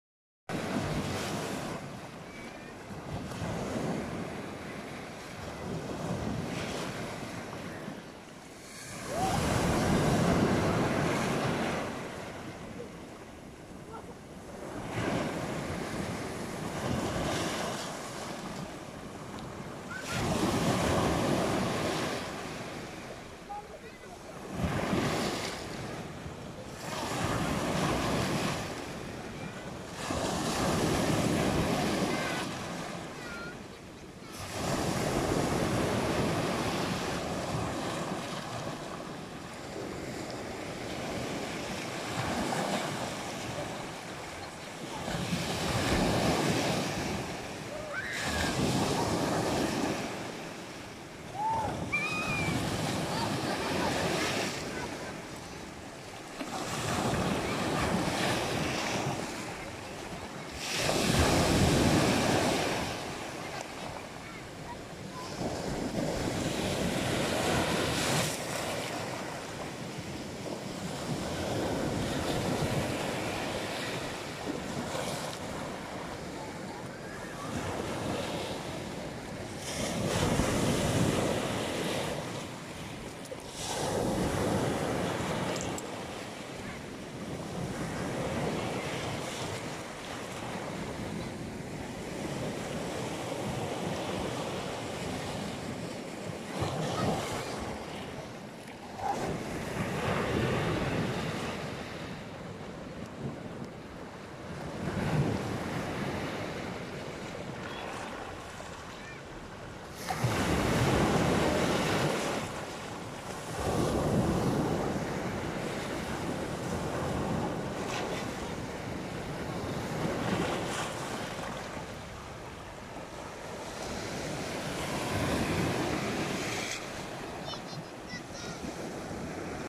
ASMR sea sounds. The seashore is beautiful today and always
ASMR sea sounds. Nature sounds video. 🎇💧👣👣👣
The seashore is beautiful today and always. What could be better?